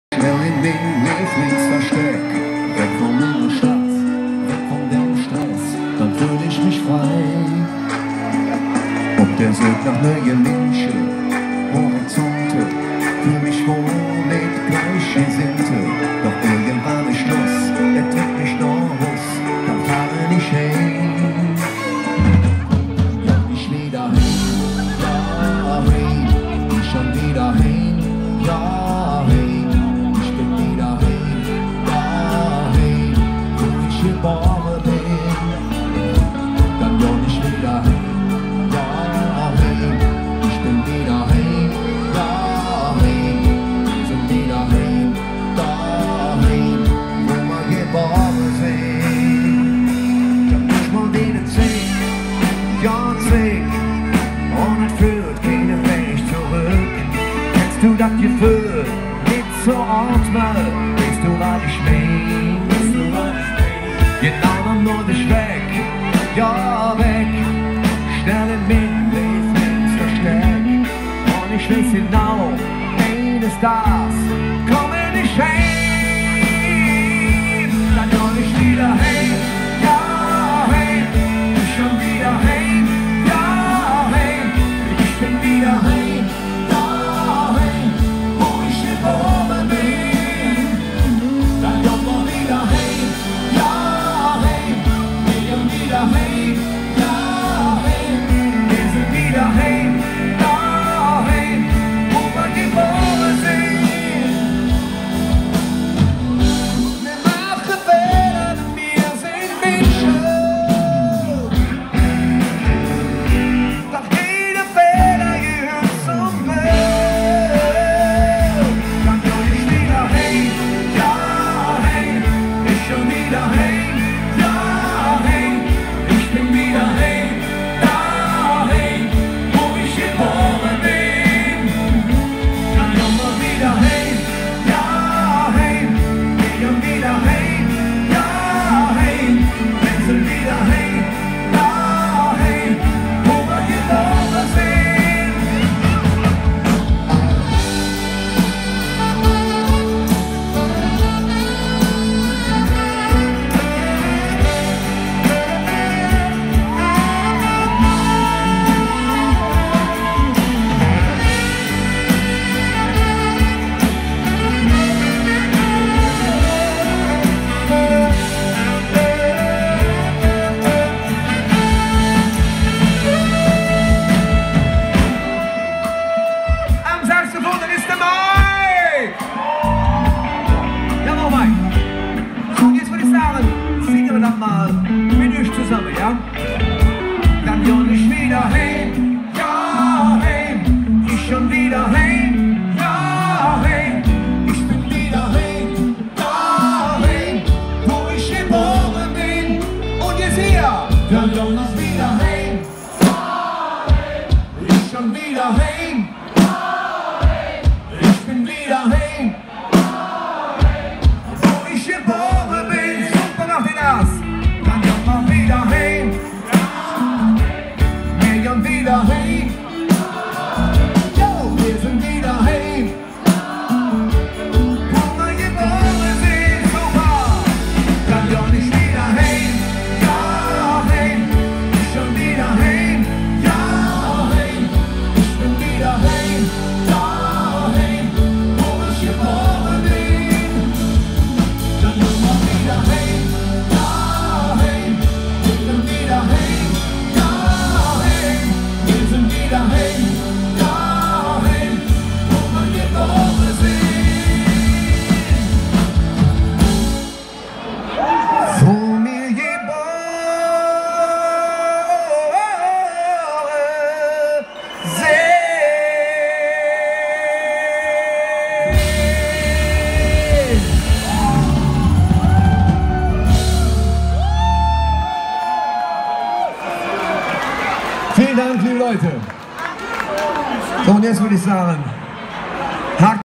Die Kölner Top Band Klüngelköpp spielte für Jacqueline Lölling.
Die Kölner Top Band - Klüngelköpp spielten für die Vize-Olympiasiegerin Jacqueline Lölling im Skeleton in Ihrem Heimatort Brachbach (Sieg) im großen Festzelt vor 2.000 begeisterten Fans!